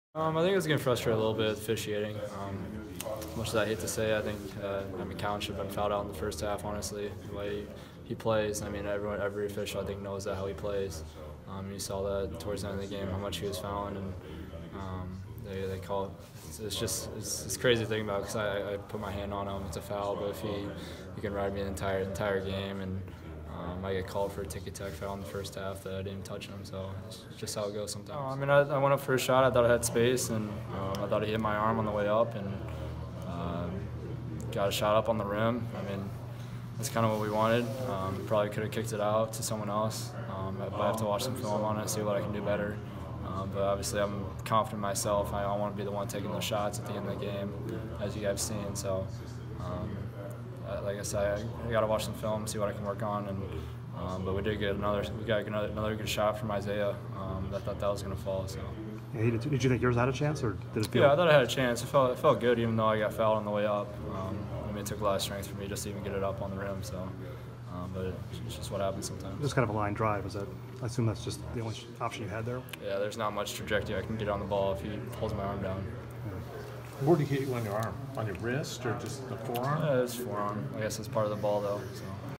Jordan Bohannon frustrated by the officiating in Iowa's matchup against Maryland
Bohannon had some pointed remarks about the officiating. Hear why he was upset, and whether he second-guesses his final shot.

Read more from the Register's Mark Emmert: Iowa's last-second shots fall short; Maryland walks away with 66-65 win